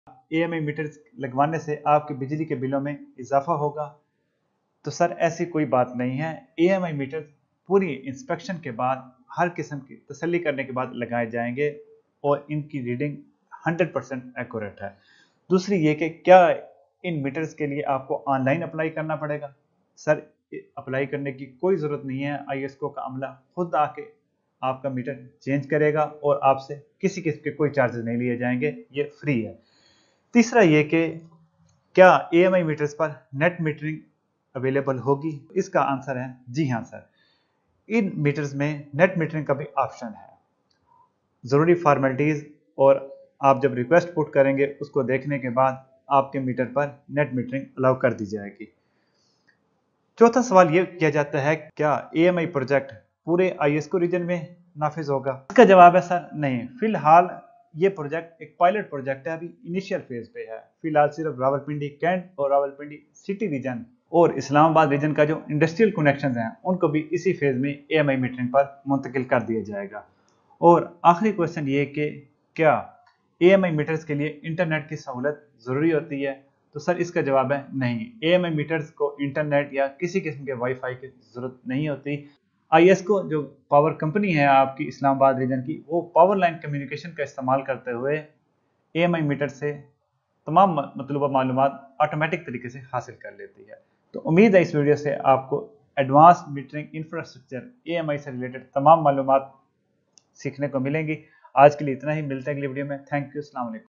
42. Advanced Metering Infrastructure|IESCO AMI Meters|Frequently Asked Questions|FAQS
Most frequently asked questions about AMI meters like charges of AMI meters, requirement of internet facility, and electricity bill for AMI meters have been addressed in this video.

🕘Timestamps:
Higher Electricity bills due to AMI Meters 00:01
How can I apply for an AMI meter 00:16
Is net metering allowed in AMI meters 00:28
Where will the AMI meters be installed 00:59
Internet Facility for AMI meters 01:20

🔎Science Speaks aims to find scientific knowledge's true meaning and application in the real world. For a full guide watch this:

.Social.